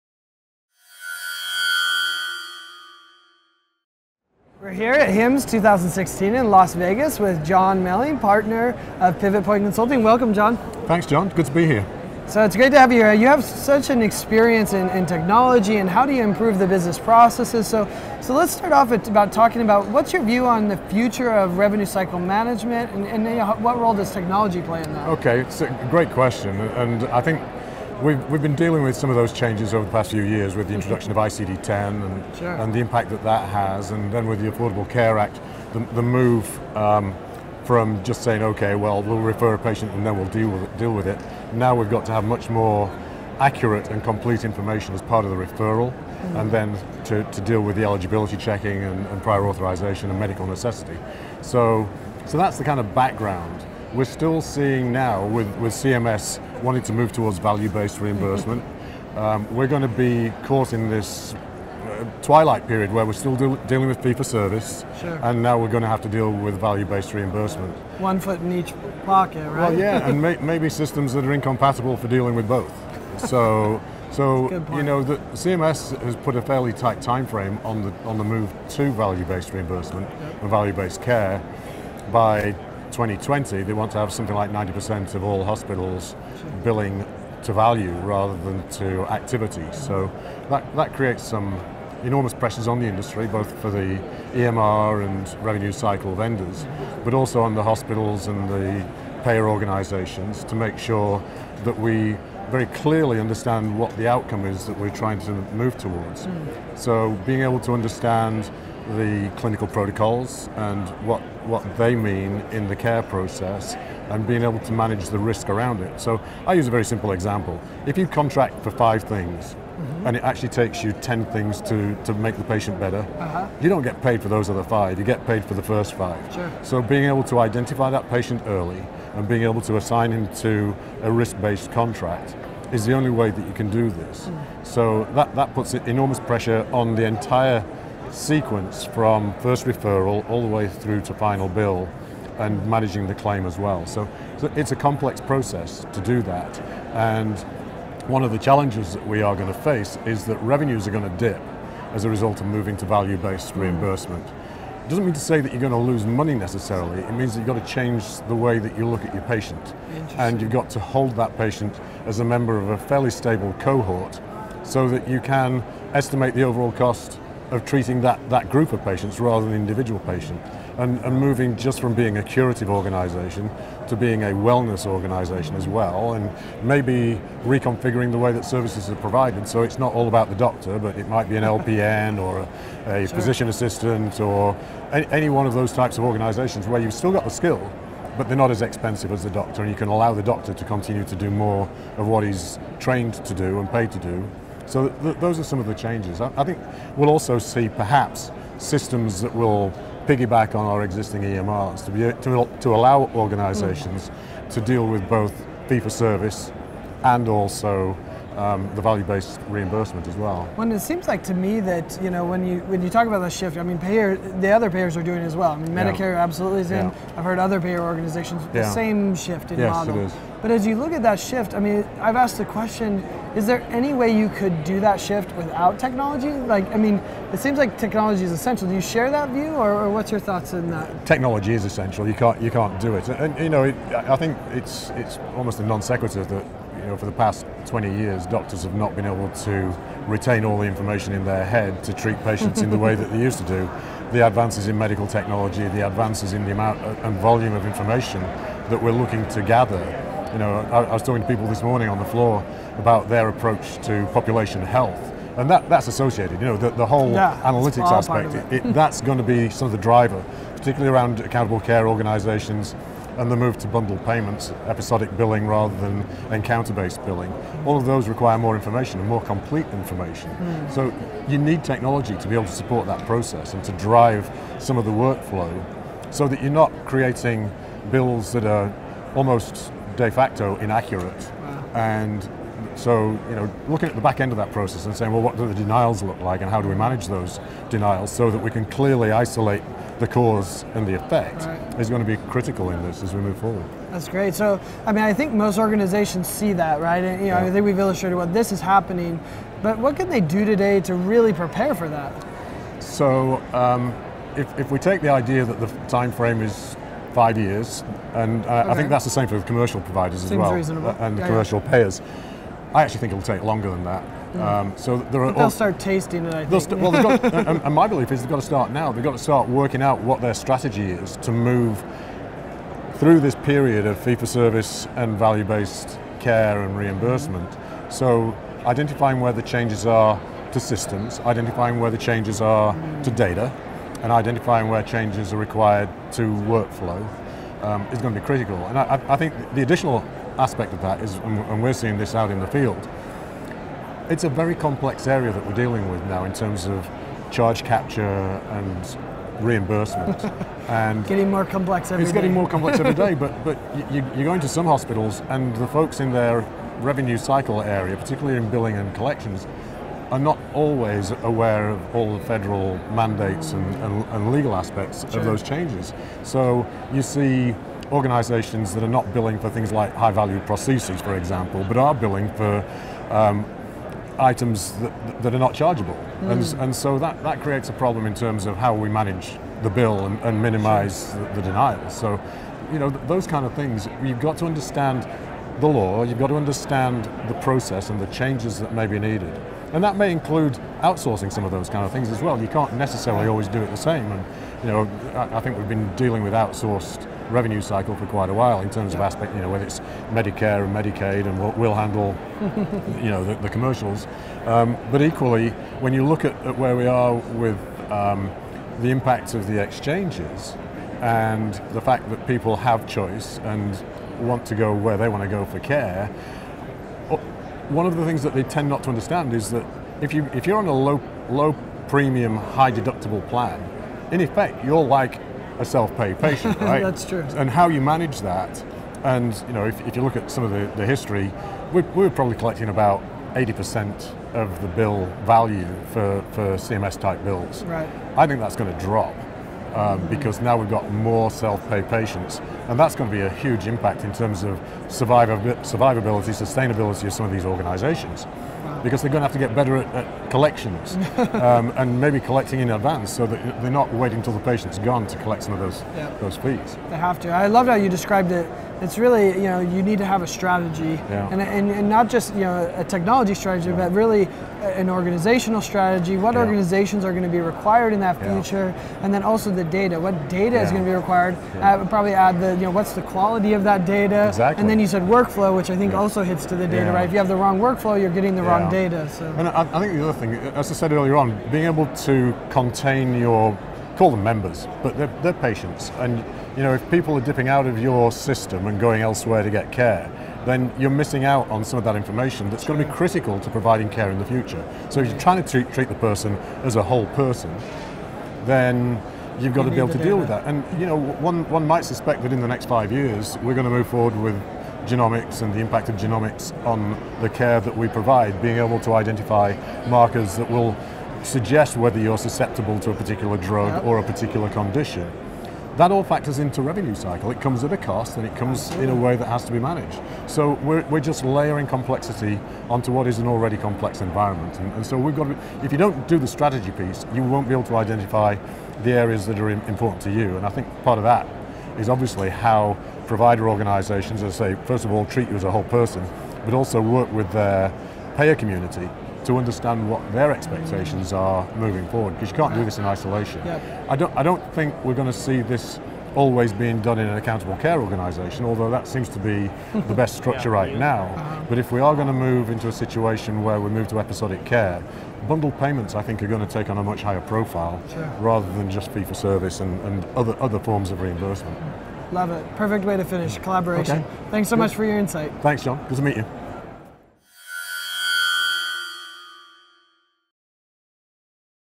What's the Future of Revenue Cycle Management and Technology with Jon Melling
While at HIMSS 2016, Healthcare Scene talked with Jon Melling, Partner at Pivot Point Consulting about changes in healthcare reimbursement and how an organization can prepare for those changes.  Plus, we talk about the role technology plays in these changes.